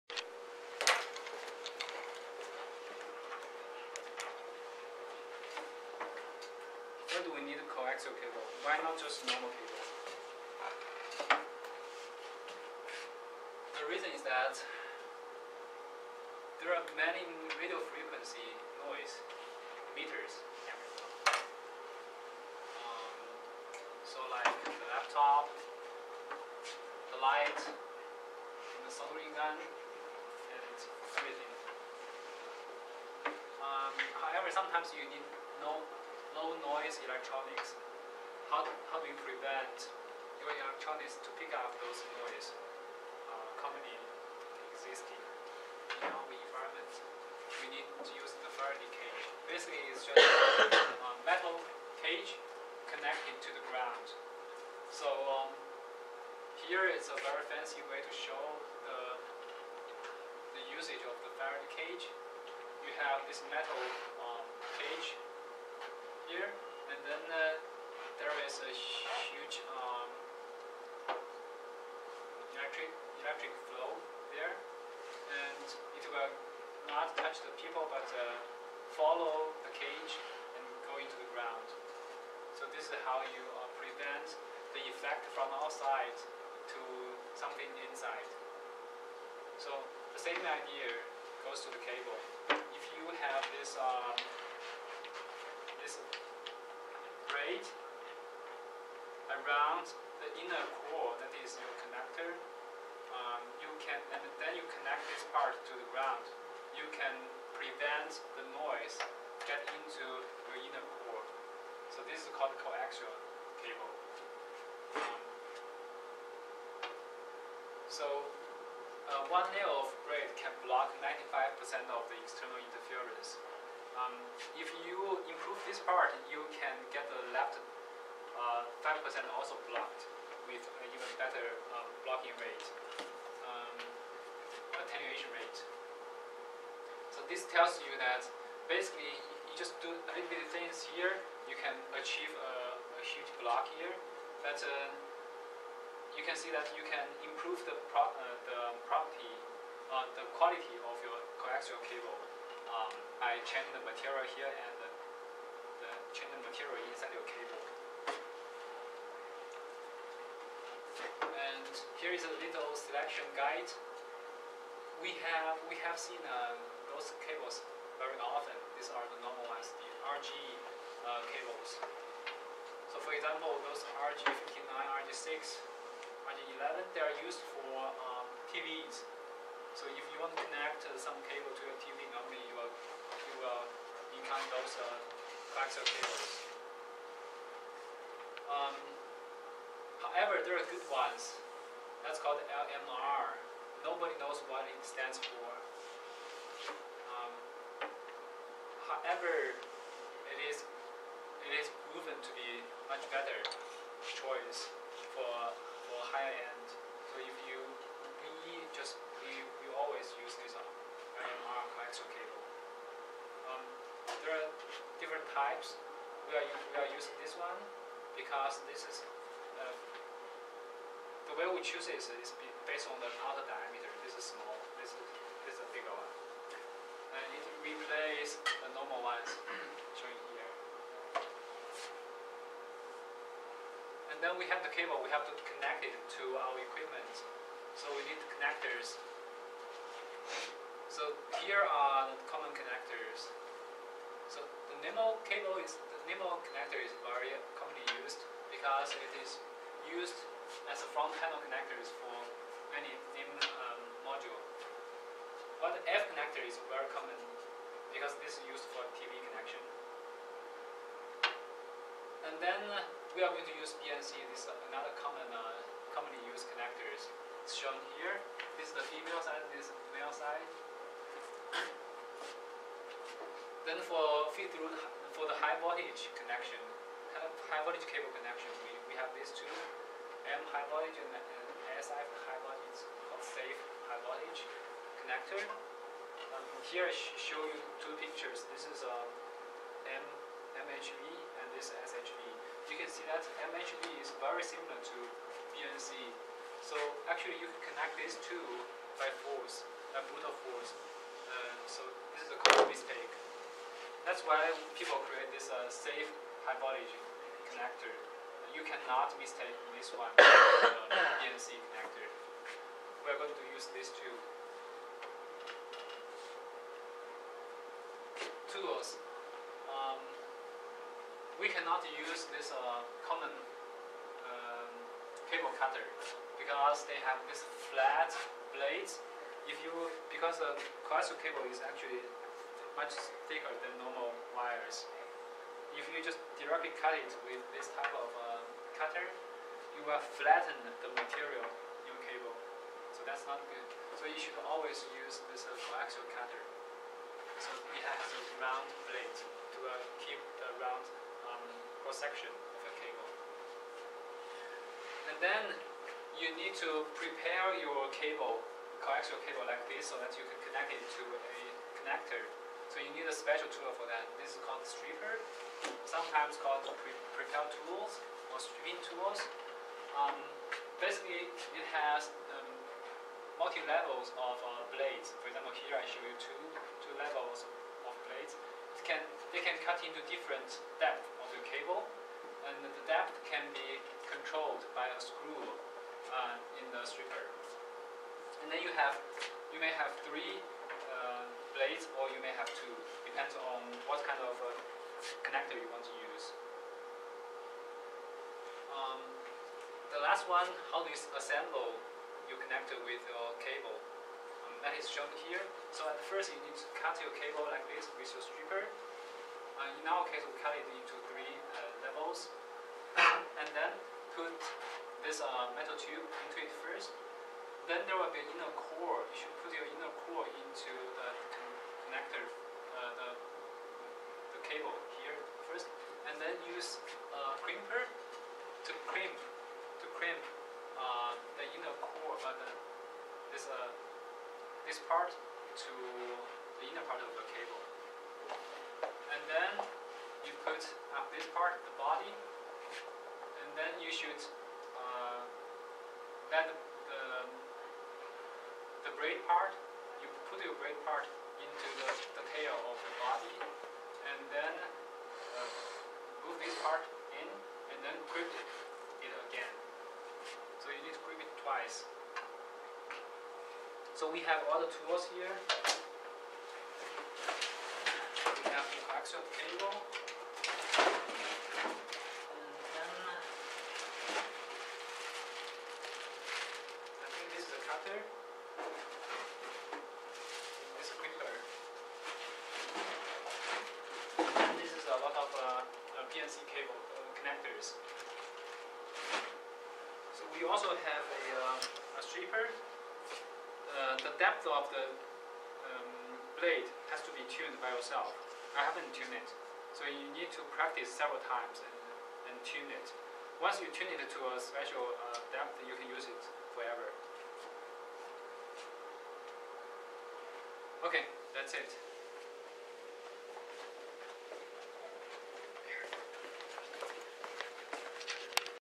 Introduction to coaxial cables
Introduction to coaxial cables in PHYS332 -Experimental Modern Physics, Spring 2015, University of South Dakota